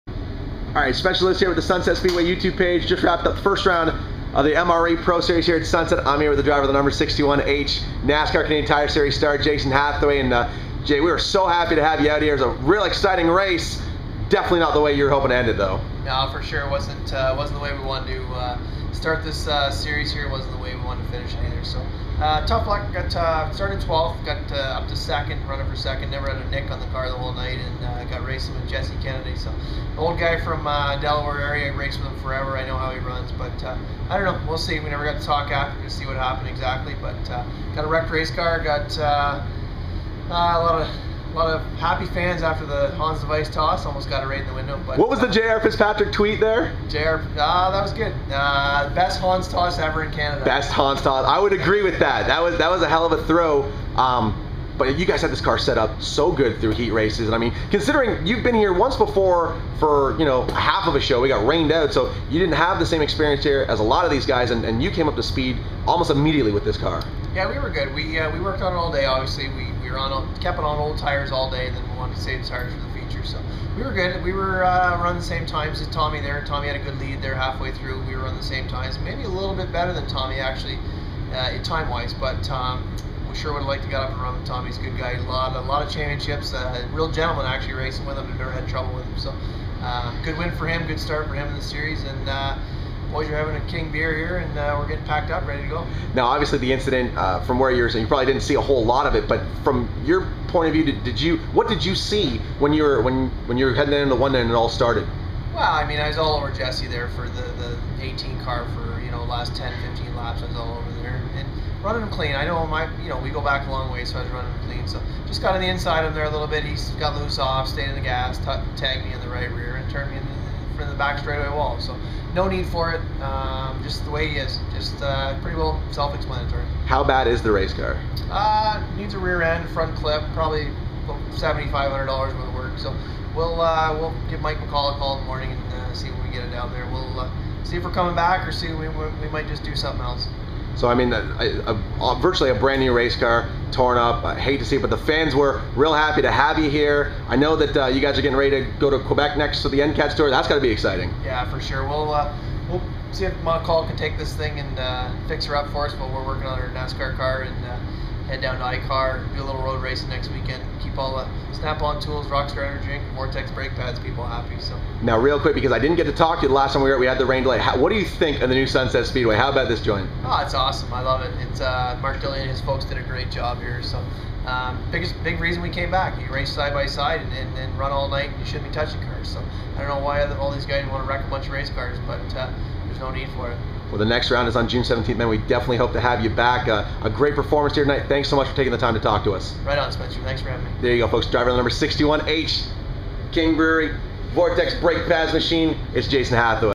2012-05-27 Sunset Speedway Jason Hathaway MRE Pro Series Round 1 Post-Race Interview
After a strong run in qualifying, NASCAR Canadian Tire Series star Jason Hathaway had his night cut short in Round 1 of the MRE Pro Series at Sunset Speedway after contact with another competitor. We caught up with Hathaway to grab his thoughts on the event as a whole, the MRE Pro Series, the new Sunset International Speedway and his plans for 2012.